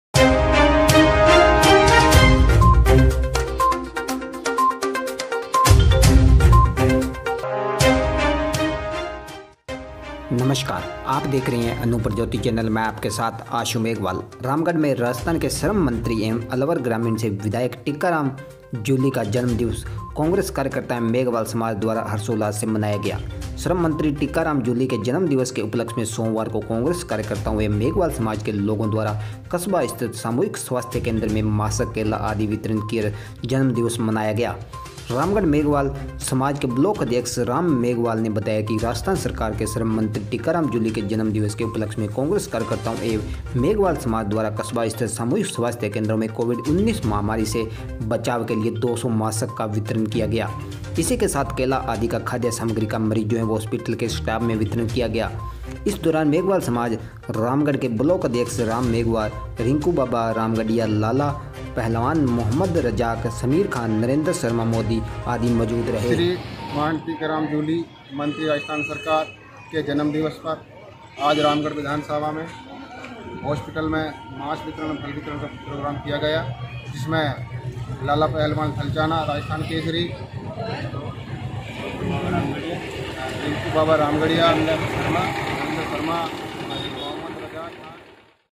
रामगढ़: राजस्थान के श्रम मंत्री टीकाराम जूली का जन्मदिन मनाया
रामगढ़ मे राजस्थान के श्रम मंत्री व अलवर ग्रामीण से विधायक टीकाराम जूली का जन्म दिवस कांग्रेसकर्ता एवं मेघवाल समाज द्वारा हर्षोल्लास से मनाया गया। श्रम मंत्री टीकाराम जूली के जन्म दिवस के उपलक्ष में सोमवार को कांग्रेसी कार्यकर्ताओं एवं मेघवाल समाज के लोगों द्वारा कस्बा स्थित सामुदायिक स्वास्थ्य केंद्र में मास्क,केला आदि वितरित कर जन्म दिवस मनाया। रामगढ़ मेघवाल समाज के ब्लॉक अध्यक्ष राम मेघवाल ने बताया कि राजस्थान सरकार के श्रम मंत्री टीकाराम जूली के जन्म दिवस के उपलक्ष में कांग्रेसी कार्यकर्ताओं एवं मेघवाल समाज द्वारा कस्बा स्थित सामुदायिक स्वास्थ्य केंद्रों में कोविड-19 महामारी से बचाव के लिए 200 मास्क का वितरण किया गया। इसी के साथ केला आदि खाद सामग्री का मरीजों एवं हॉस्पिटल के स्टाफ में वितरण किया गया। इस दौरान मेघवाल समाज रामगढ़ के ब्लॉक अध्यक्ष राम मेघवाल, रिंकू बाबा रामगढ़िया, लाला पहलवान, मोहम्मद रज्जाक ,समीर खान, नरेंद्र शर्मा मोदी, आदि मौजूद रहे।